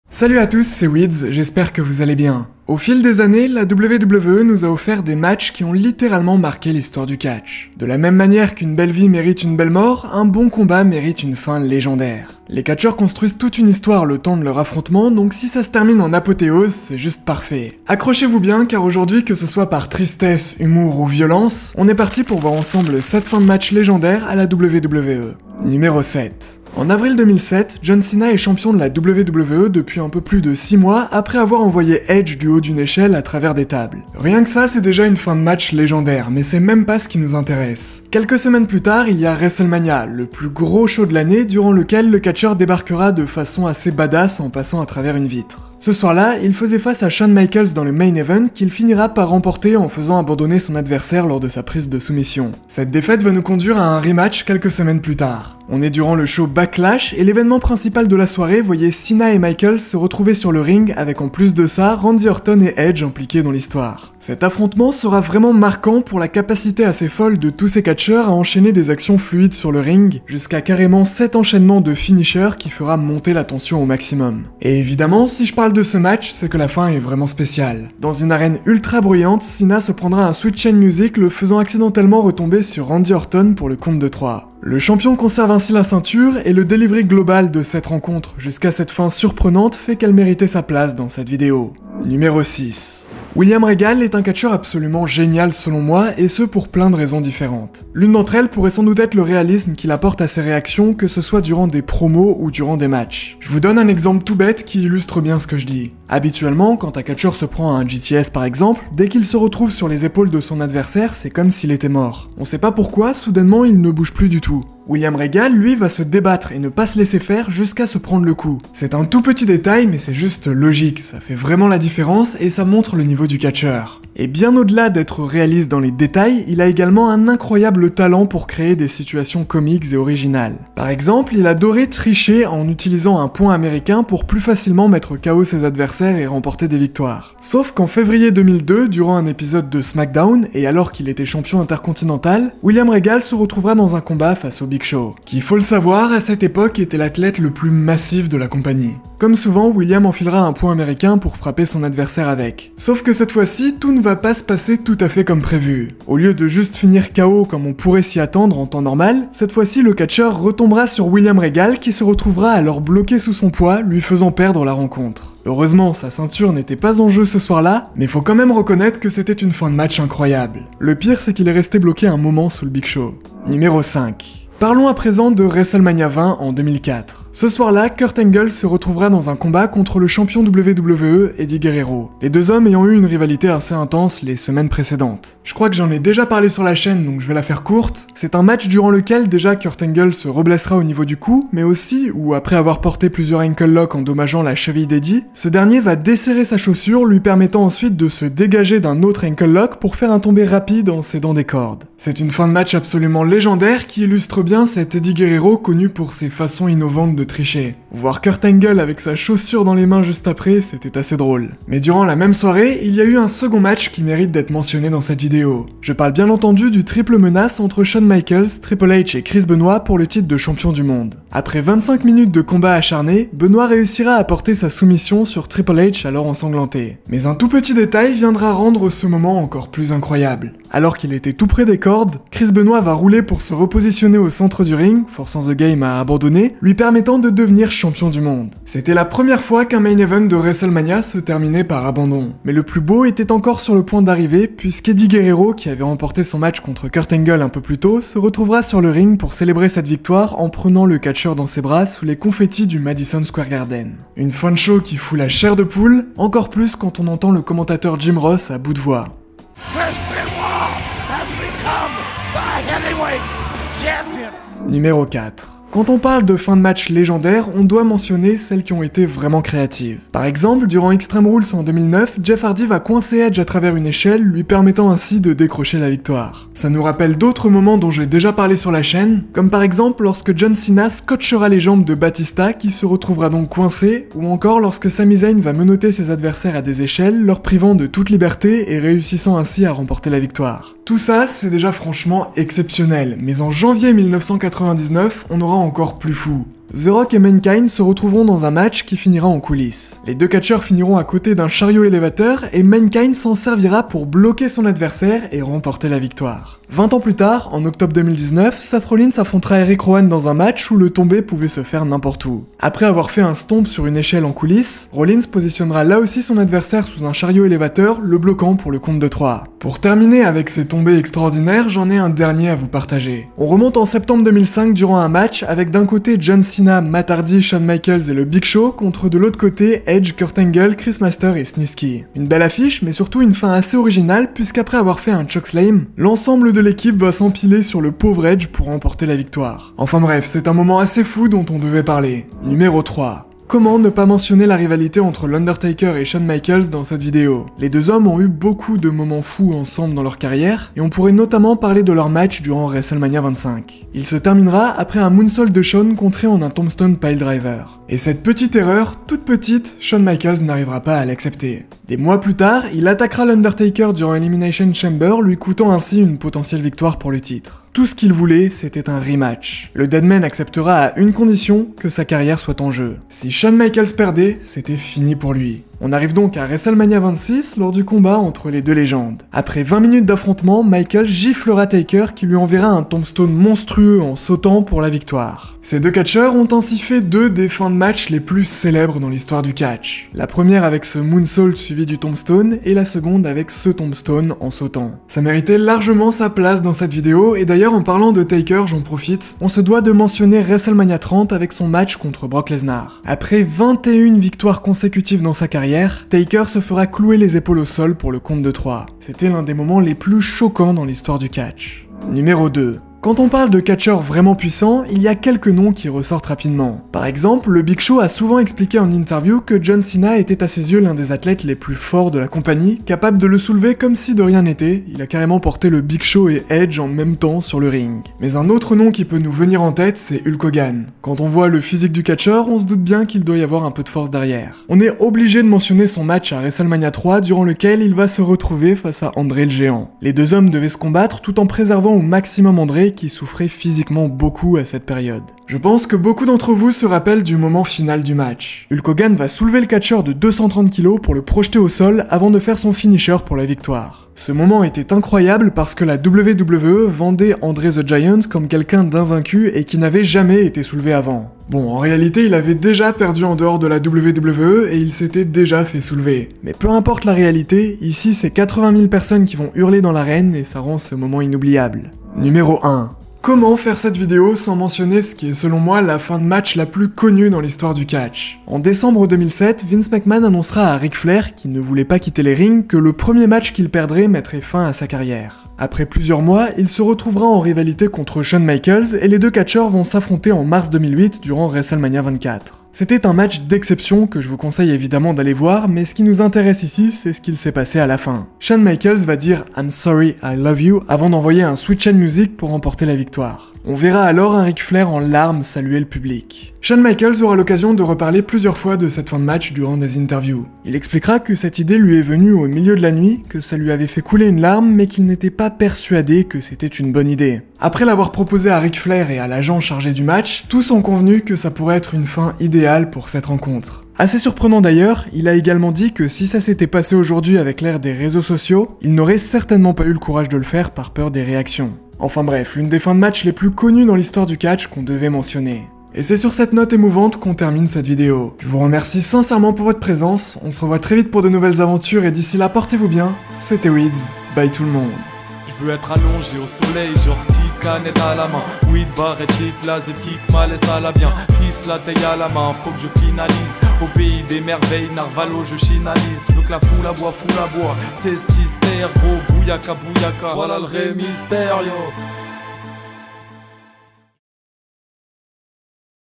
7 fins de match LÉGENDAIRES à la WWE !
Salut tout le monde, en retard mais toujours présent : on est partis cette semaine pour voir 7 fins de match incroyables à la WWE. N'hésitez pas à partager la vidéo si ça vous a plu.

Merci pour tout, vraiment ❤️
On se revoit très vite sur la chaine ! 🤙🔥

Weedze